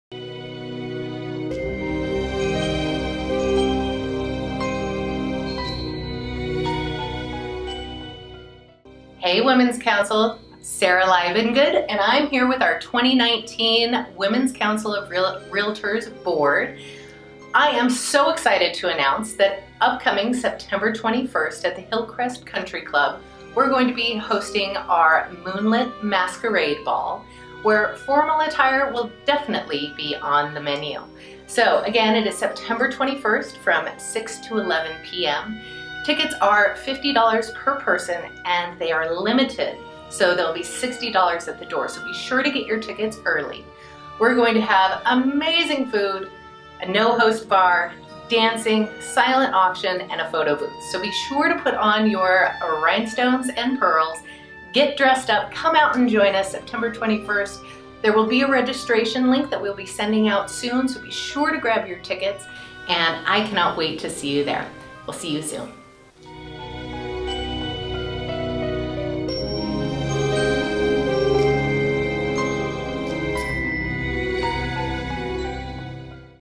Moonlight Masquerade Ball!
Women's Council Members we are proud to announce our Fall Event!  Please join us September 21st from 6-11 at Hillcrest Country Club for an evening to remember!!!  Call Sarah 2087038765 for more information!